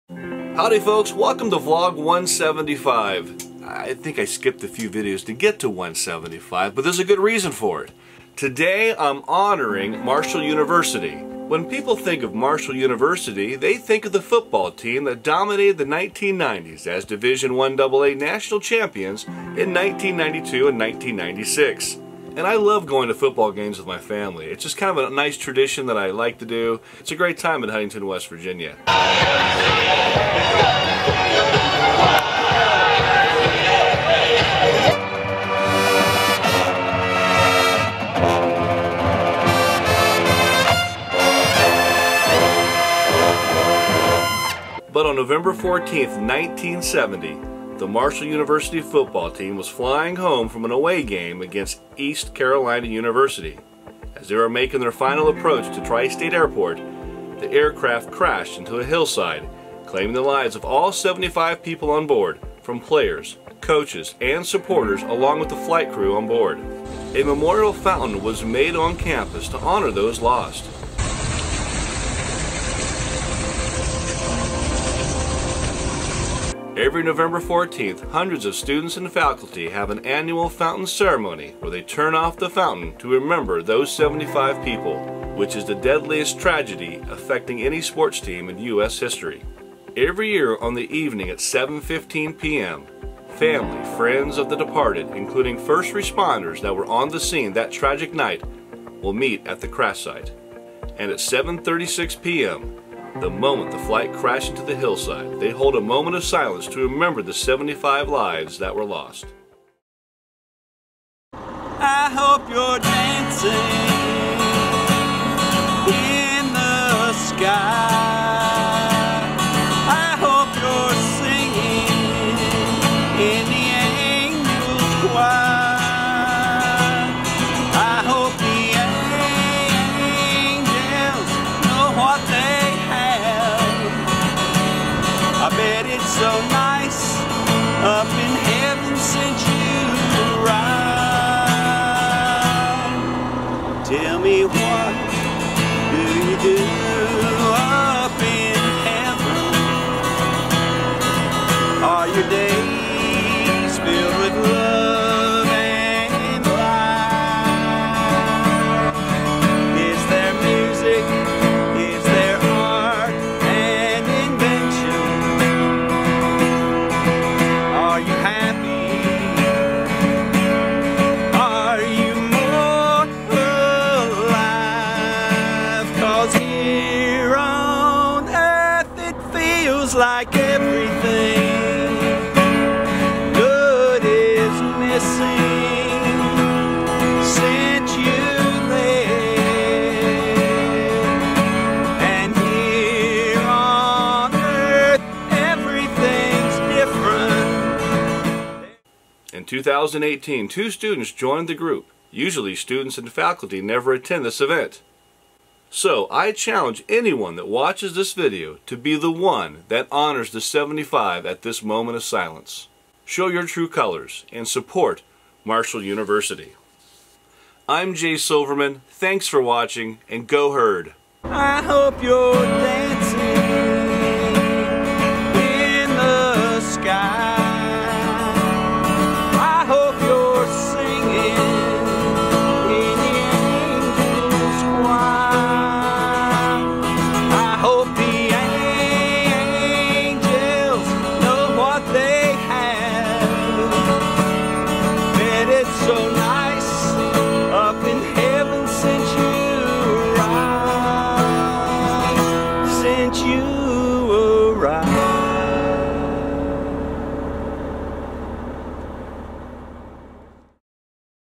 Honor the 75 - Vlog 175
November 14, 1970, the Marshall University Football team fatally crashed into a hillside on approach to the Tri-State Airport. All 75 people on board lost their lives. Today, Marshall University has a ceremony on campus in their memory but not many are aware of the "moment of silence" that is dedicated at 7:36pm at the crash site. This video is about that event and the challenge to you to Honor the 75.